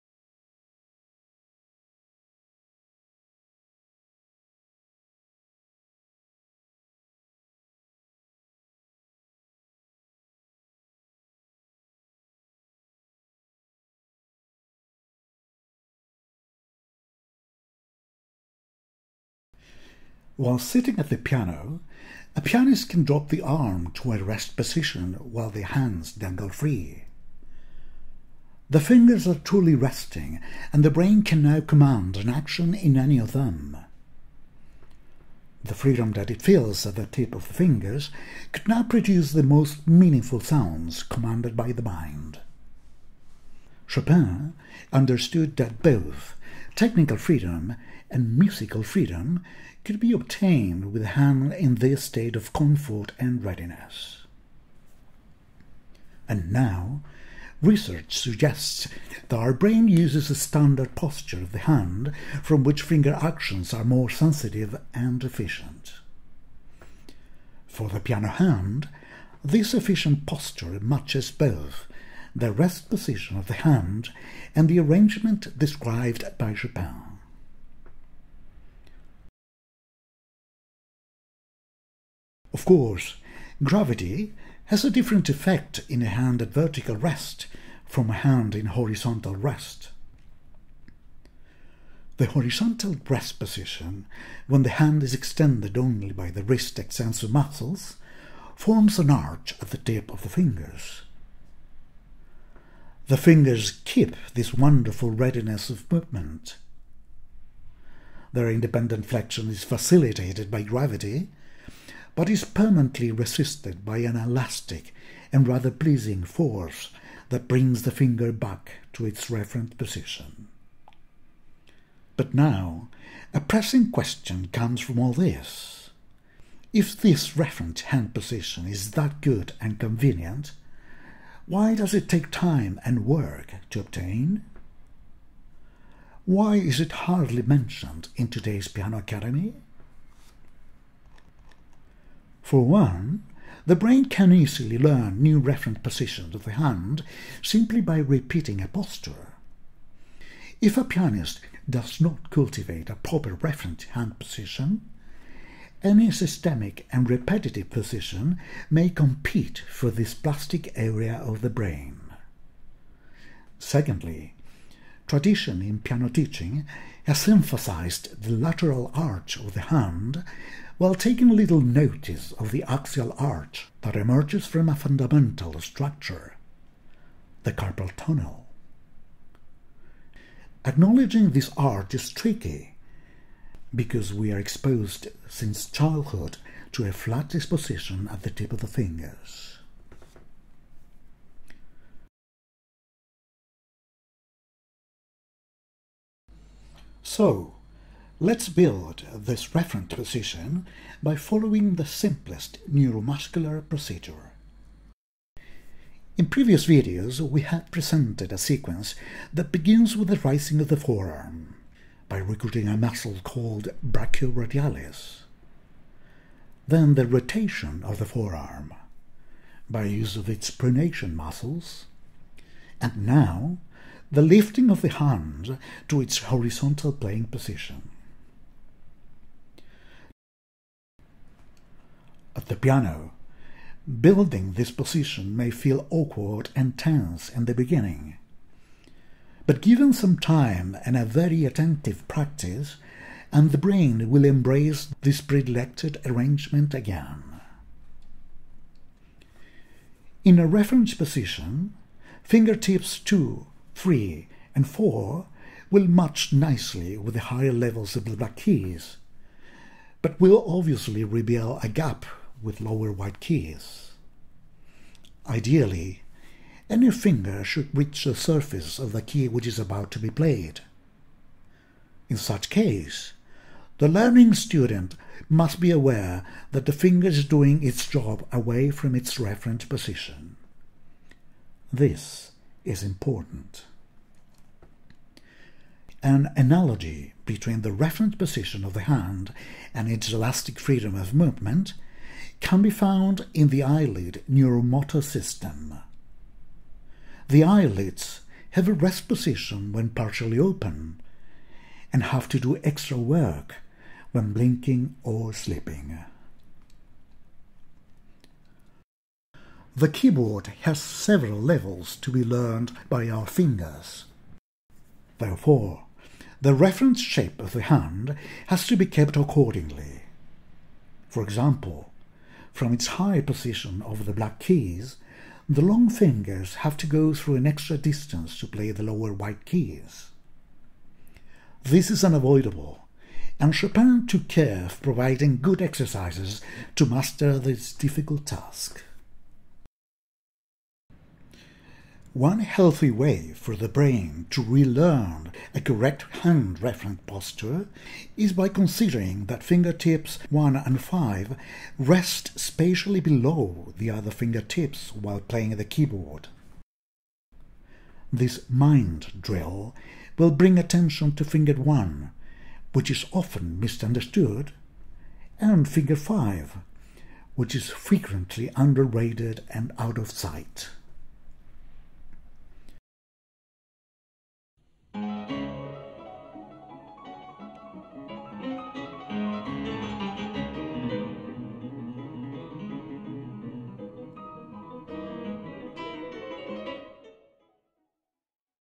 The Chopin Method: Piano lesson 4. The hand and the keyboard. Part two.
This session will explore the the real meaning of a referent hand position for piano playing. Here you will learn why a referent hand position differs from a C Major surface of the keyboard. And more.